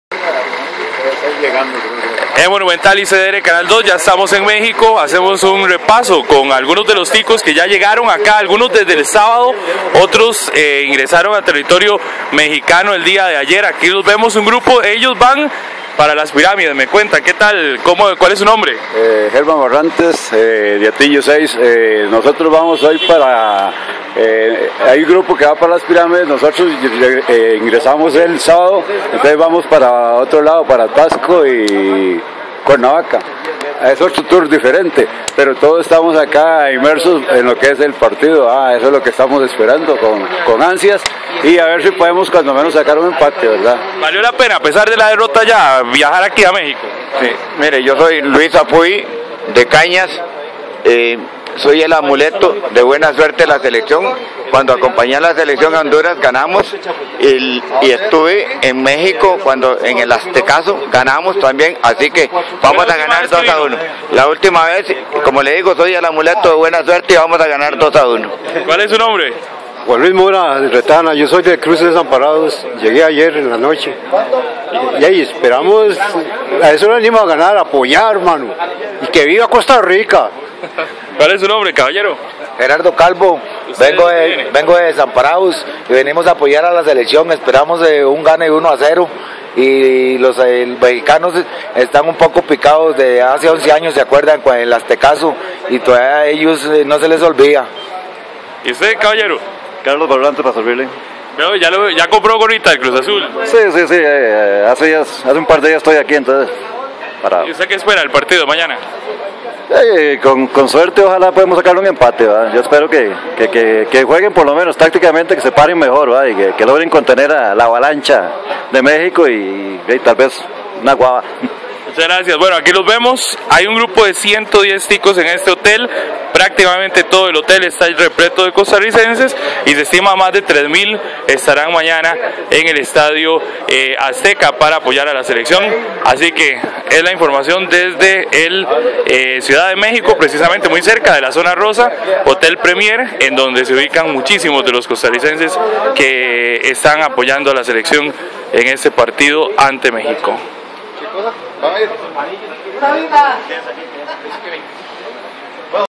Radio Monumental: ticos en México
Se estiman más de 3 mil mañana en el Azteca